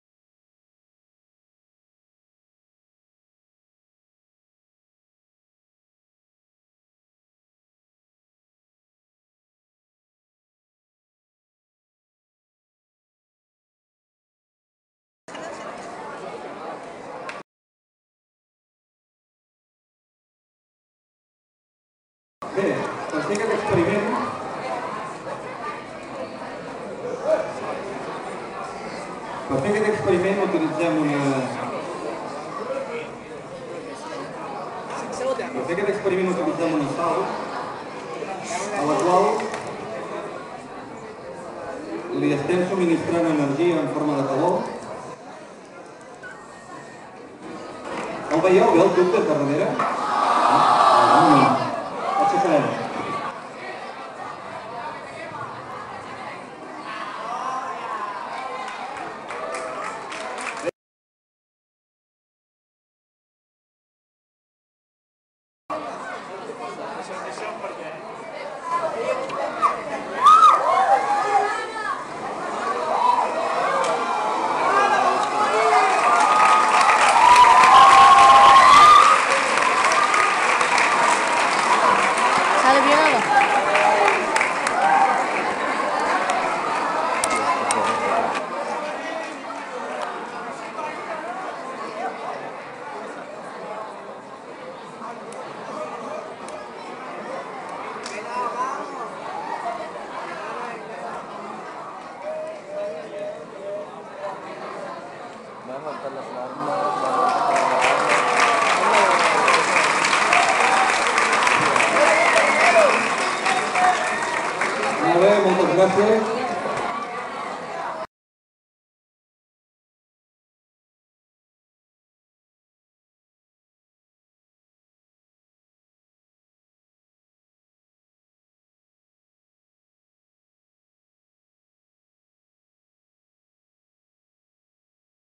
xuxe hostalric
Experiment de l'oxidació del sucre d'un caramel mitjançant clorat de potassi.
Reacciona... explota! a l'IES Vescomtat de Cabrera. Hostalric, 19/02/10.